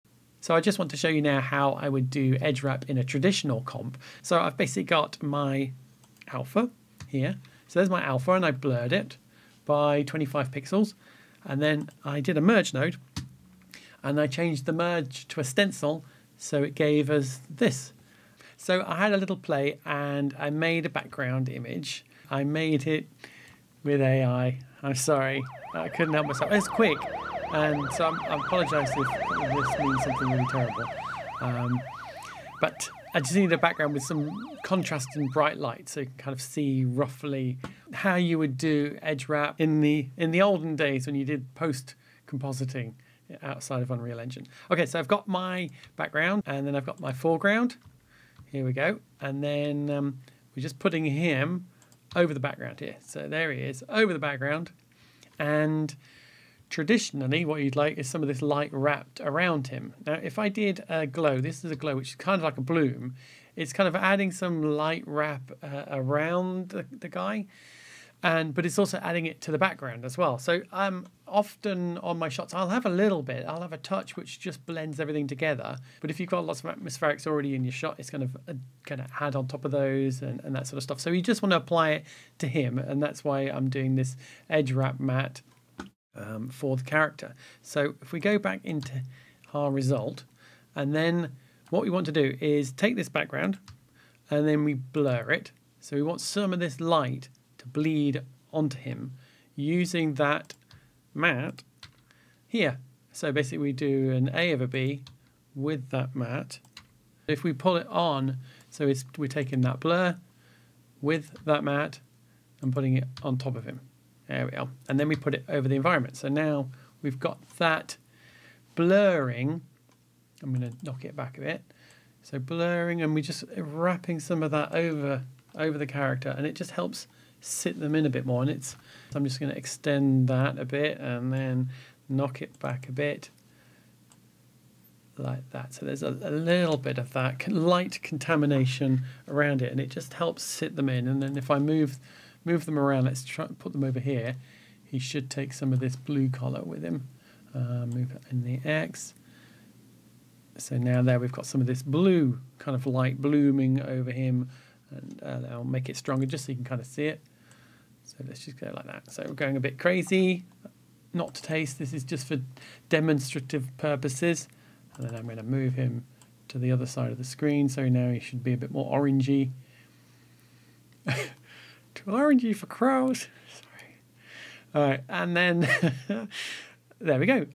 Create traditional 2D Edge Wrap in DaVinci Resolve / Fusion
How to create traditional 2d Edge Wrap in DaVinci Resolve / Fusion - taken from my long form video about mimicking edge wrap in unreal engine which uses this principle. Thought it might make an interesting standalone, thanks.